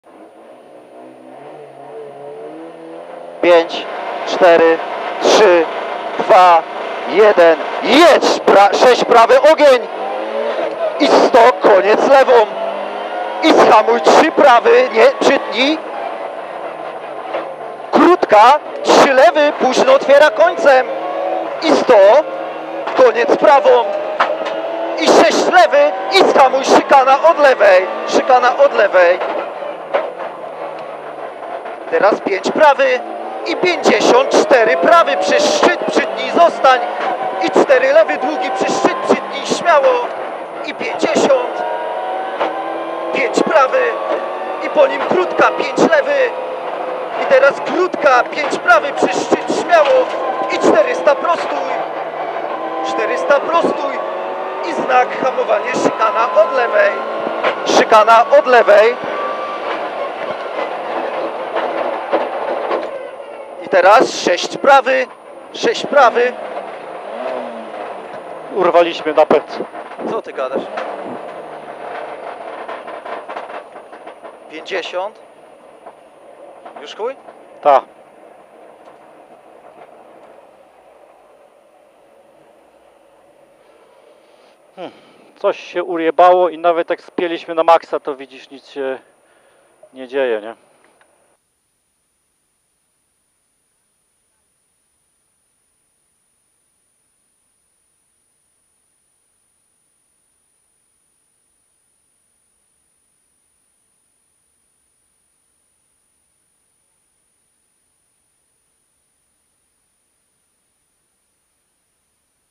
BSK-TECH Rajd Wikinga 2020 - Bati / Piotrek Lancer Evo IX ON BOARD OS 1
Scieło śruby przełożenia głównego w skrzyni :(